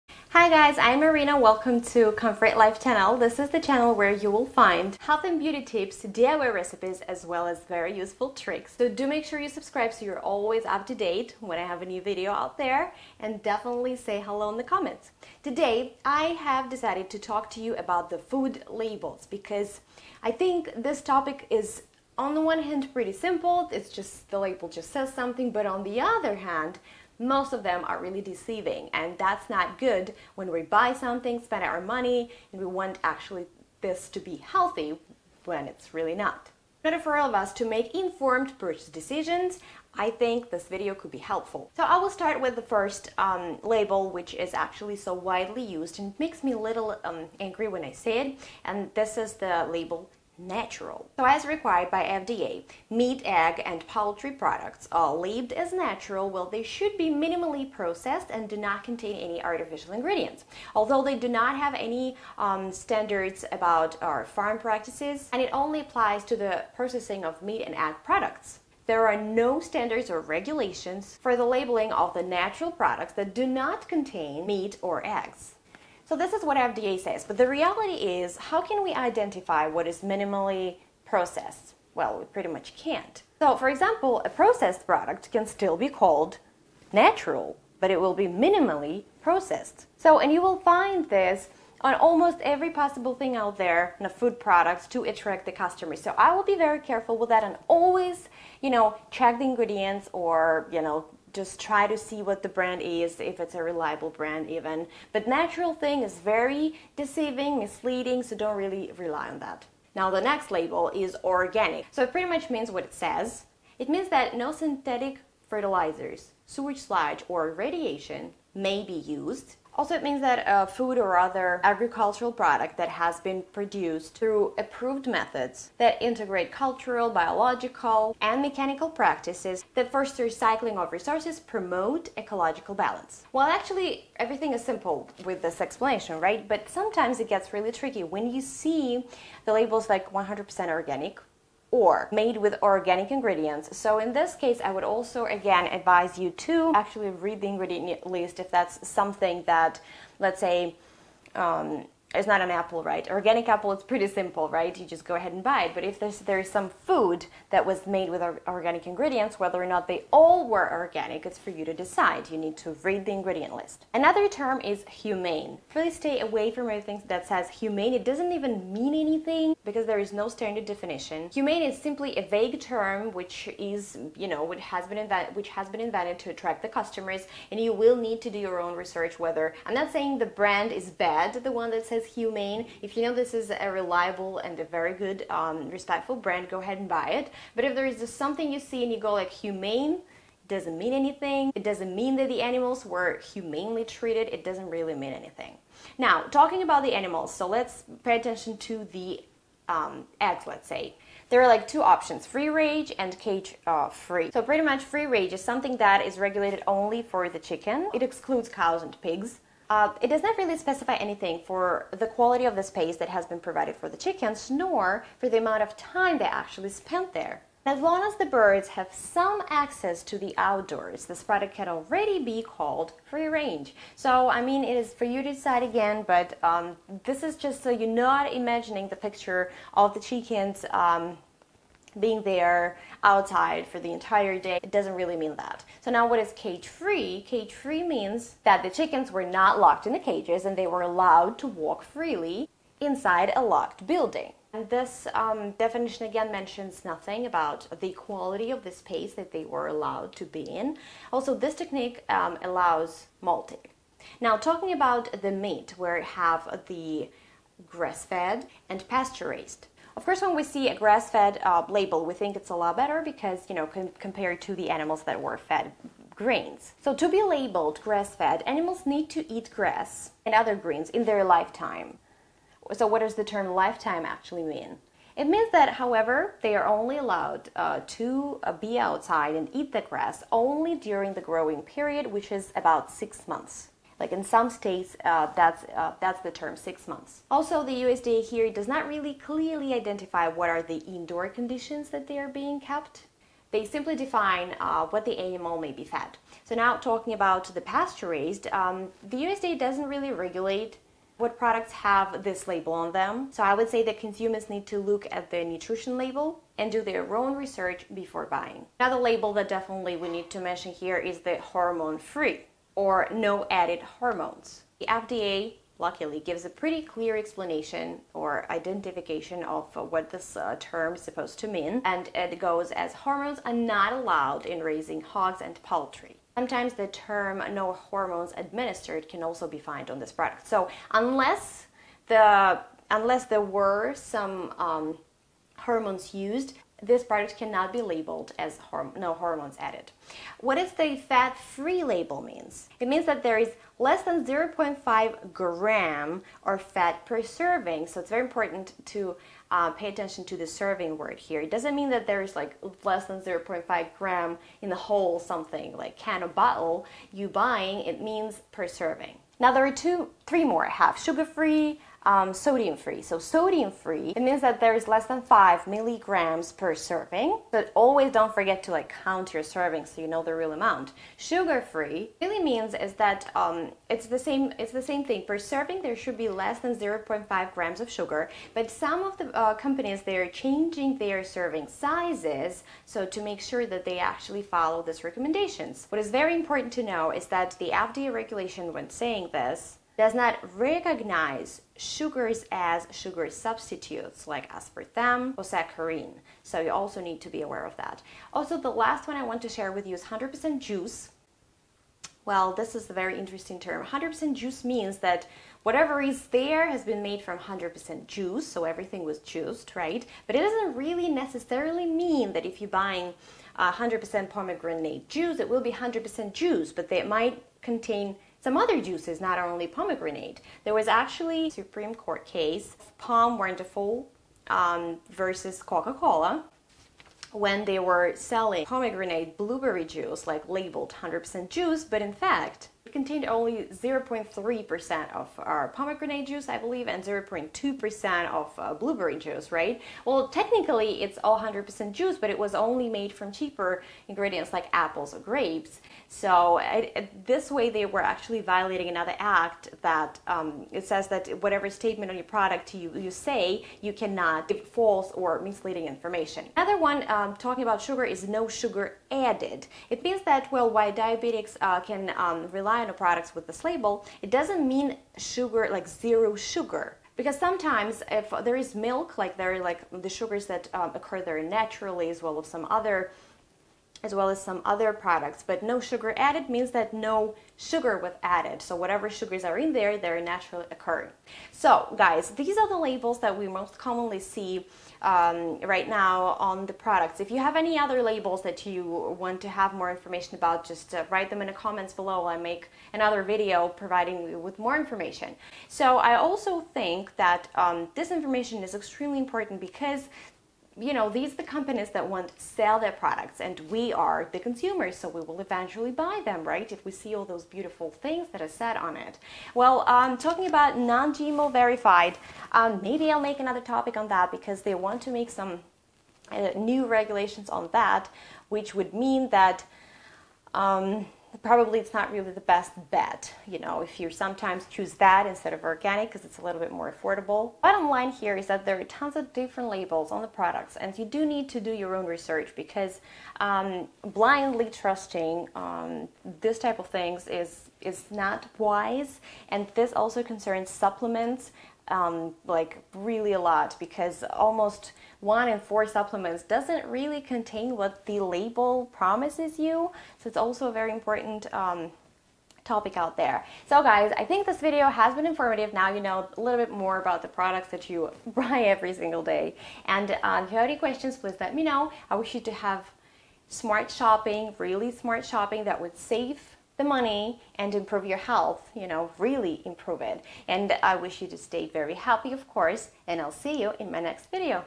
What is that food label really saying How to understand Food Labels
Are you confused about food labels? Does the information seem misleading? Check out this video to learn everything you need to know about the most common food labels and shop like a pro!

99% Natural 0:48
USDA Organic 1:50
Humane 2:48
Free Range Eggs 3:25
Cage-Free Eggs 3:56
Grass-Fed Meat 4:20
Pasture-Raised 4:57
Hormone-Free 5:11
Fat-Free 5:42
Sodium-Free 6:05
Sugar-Free 6:13
100% Juice 6:40
No Sugar Added 7:38

🔴 MY COURSE: " The complete Guide to Facelift Massage and Lymph Drainage"
⚡️ My ETSY Store "Vintage Love NY"